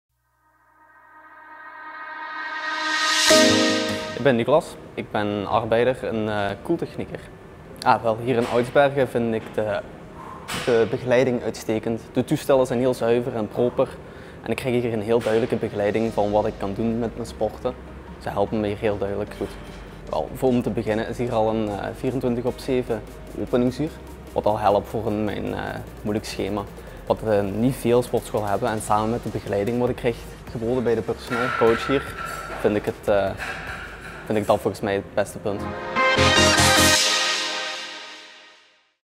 At My Gym Oudsbergen interview Nikolas
Interview met een lid van sportschool At My Gym in Oudsbergen.